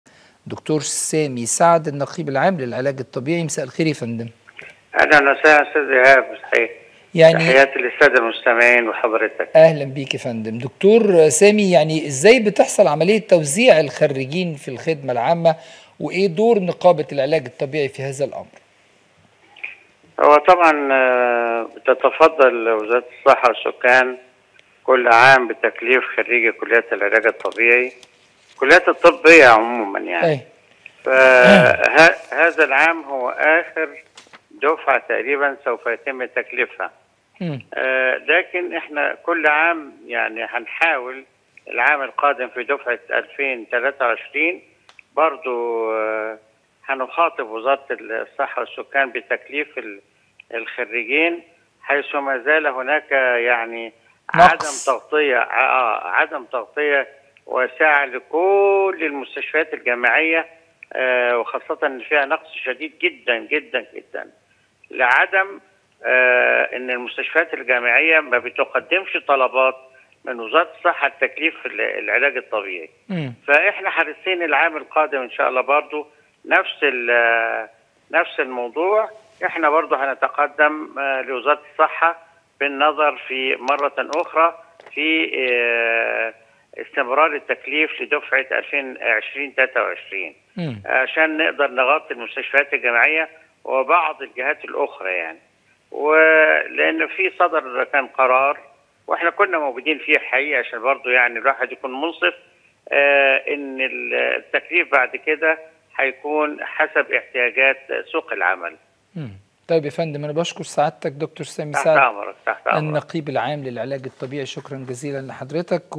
د. سامي سعد عن تكليف خريجي كليات العلاج الطبيعي ٢٠٢٢ في هذا العام آخر دفعة، لكن سنحاول حسب الاحتياج
د. سامي سعد عن تكليف خريجي كليات العلاج الطبيعي ٢٠٢٢:
 في هذا العام آخر دفعة، لكن سنحاول حسب احتياج سوق العمل.